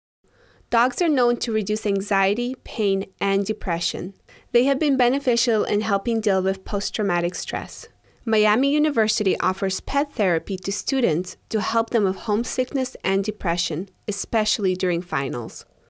Dogs and Puppies Help With Anxiety, Stress and Depression
"Dogs are known to reduce anxiety, pain, and depression. They have been beneficial in helping deal with Post Traumatic Stress. Miami University offers pet therapy to students to help them with homesickness and depression, especially during finals." .

1)This video has no negative impact.
2)This video is also for teaching & Informational AND Entertainment purposes.
3)It is not transformative in nature.